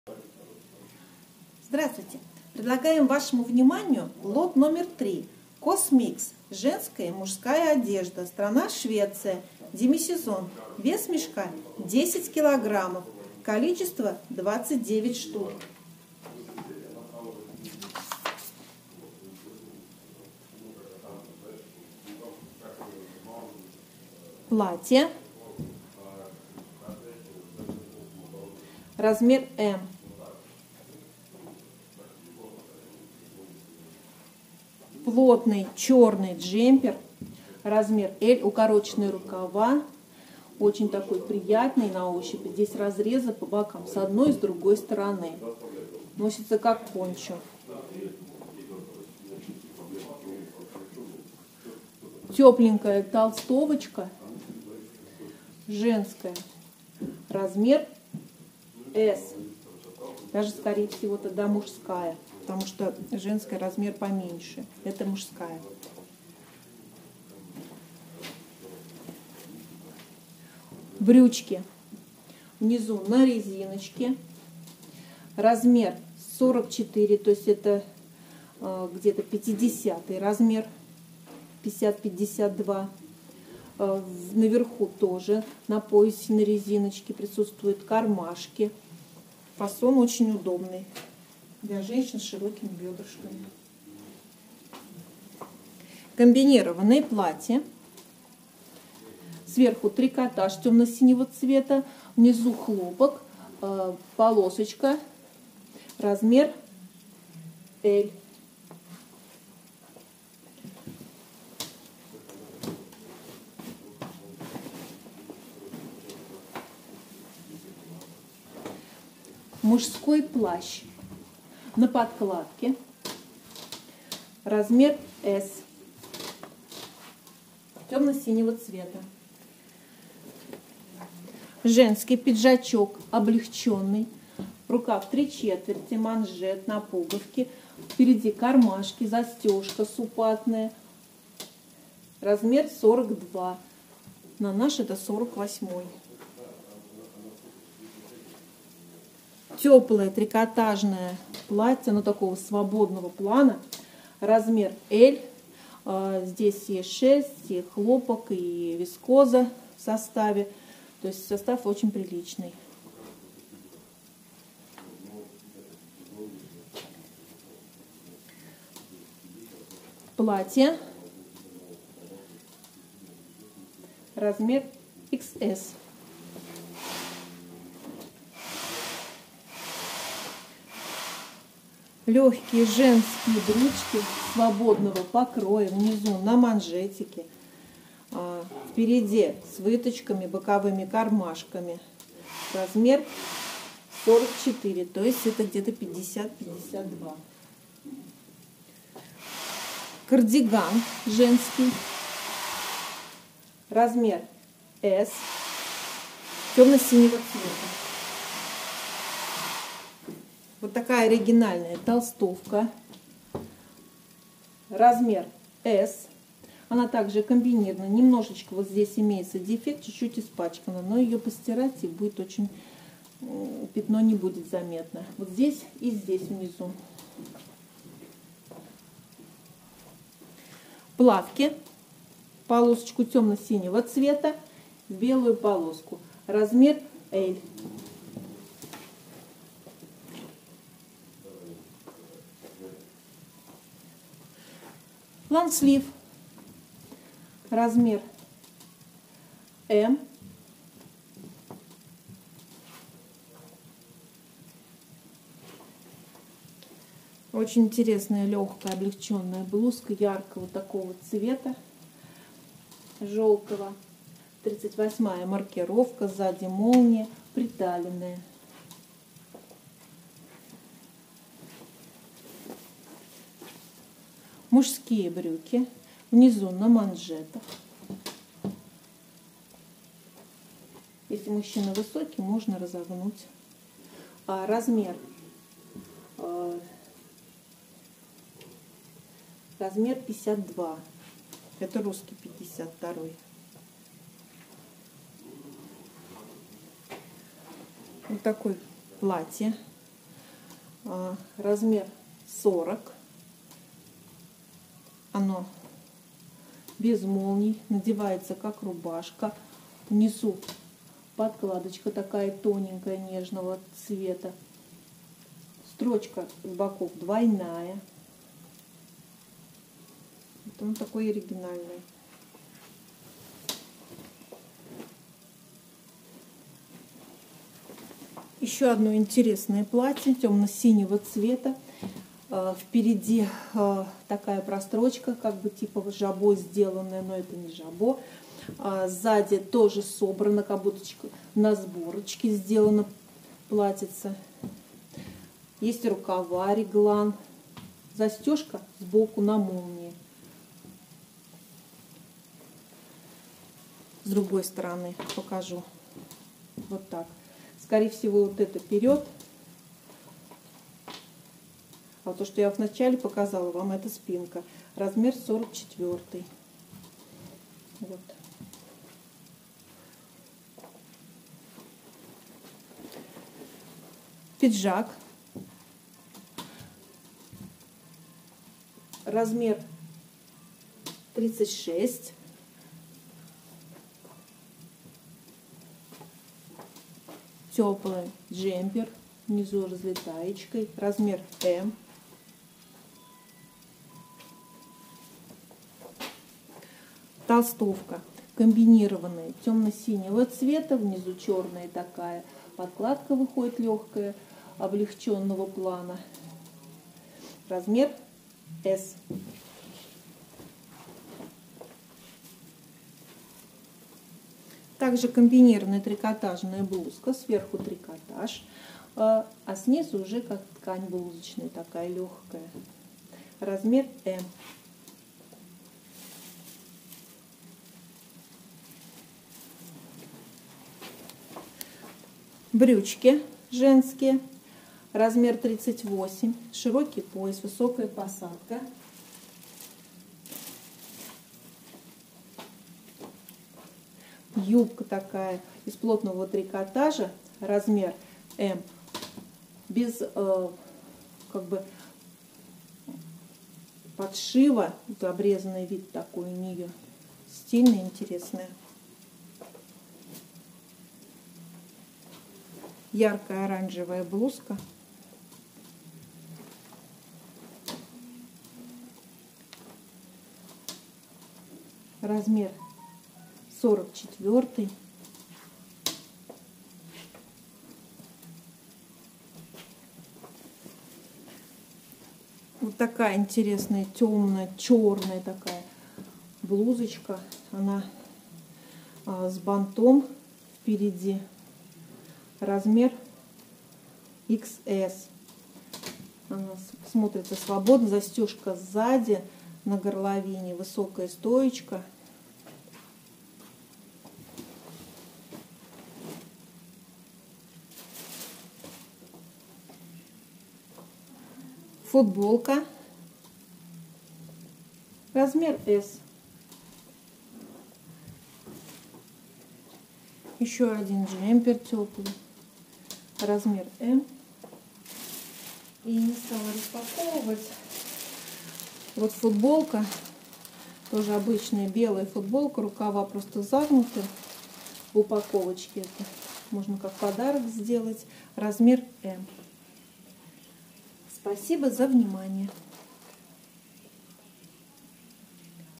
COS mix 3, сток одежда оптом
COS mix (Лот №3)
Женский,мужской микс (сток)
Страна: Швеция
Демисезон
Цена за кг.: 43.03 евро
Цена за мешок: 430.3 евро
Цена за ед.: 14.83евро